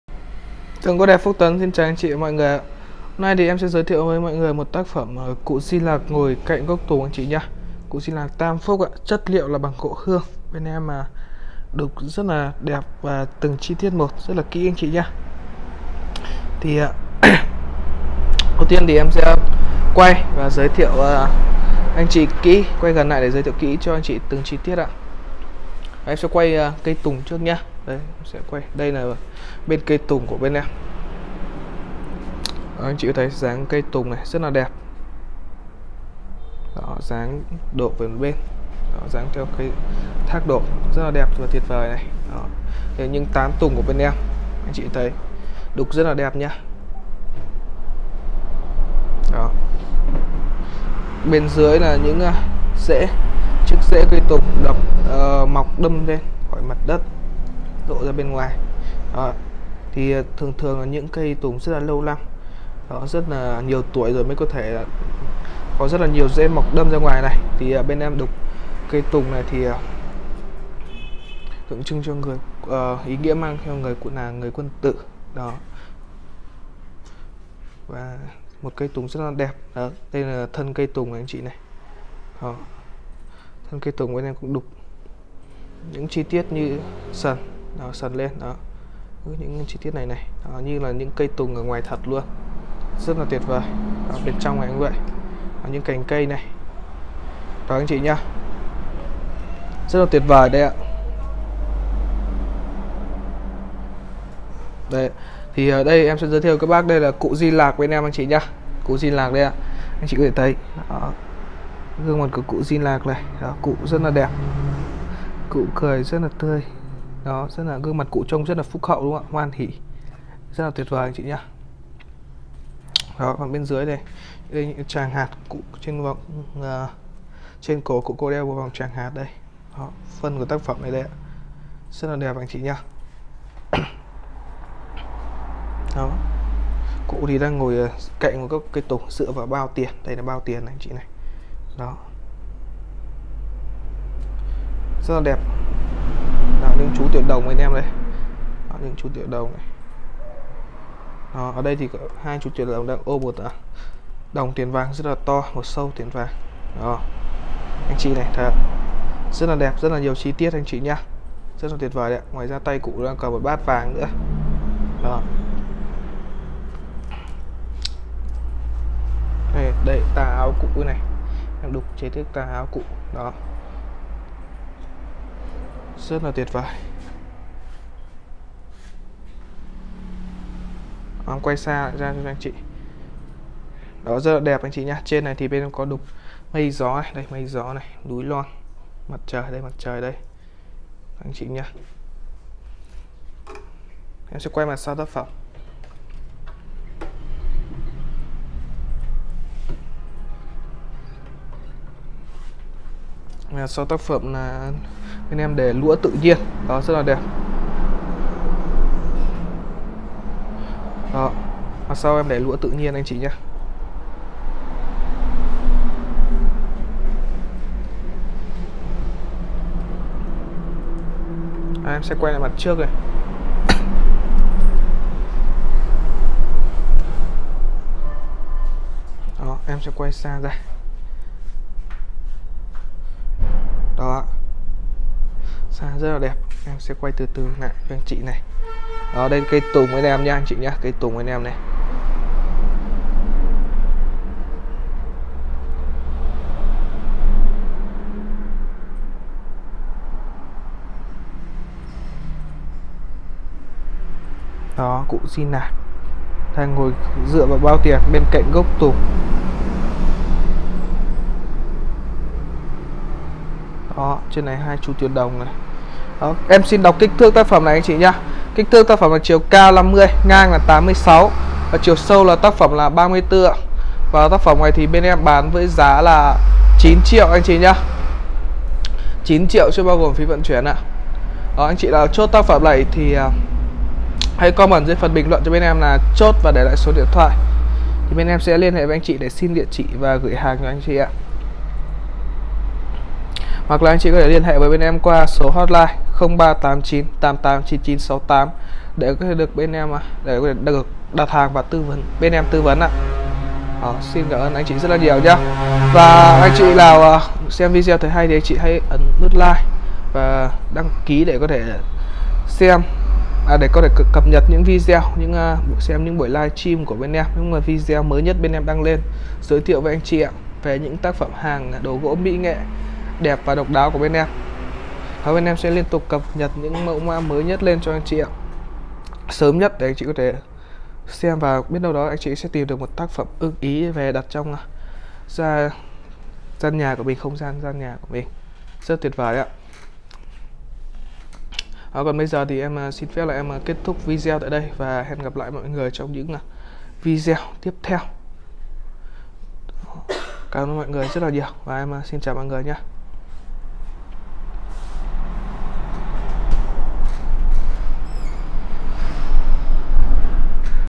DI LẶC ngồi bên cạnh gốc tùng gỗ HƯƠNG TA giá rẻ chỉ 9tr5
CỤ DI LẶC HÀNG TO NẶNG DẦY GỖ VÀ TINH LÕI, cụ ngồi bên cạnh gốc cây tùng và có 3 chú tiểu đồng.